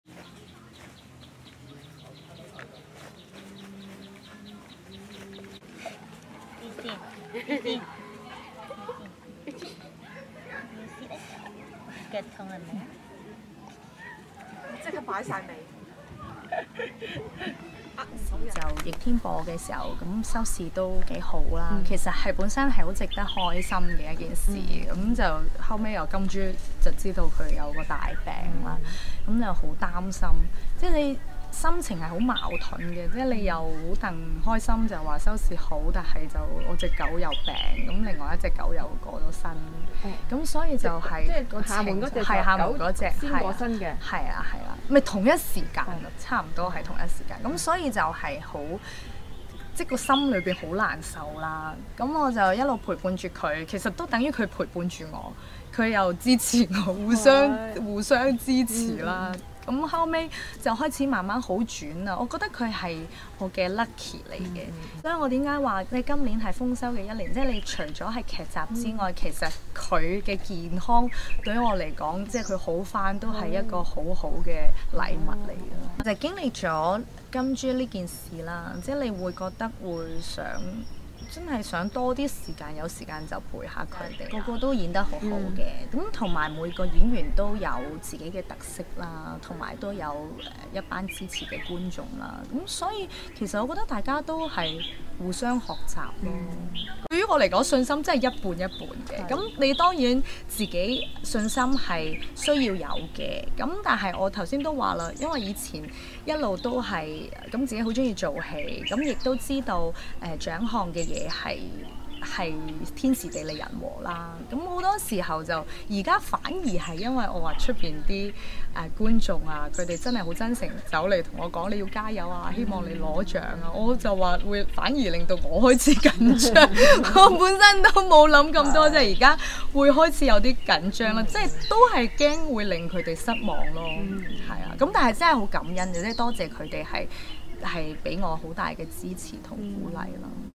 林夏薇奪視后信心一半半
林夏薇憑劇集《七公主》及《逆天奇案》兩部劇，分別角逐「最佳女主角」、「最受歡迎電視女角色」及「最受歡迎電視拍檔」，薇薇直言對角逐視后信心一半半，作為演員比較被動，她盡力演好每個角色，而每次都好享受過程，角色有發揮、劇集好成績、觀眾鍾意已經不容易；最重要愛犬「金豬」健健康康，她形容2021年是豐收之年，特別有意義！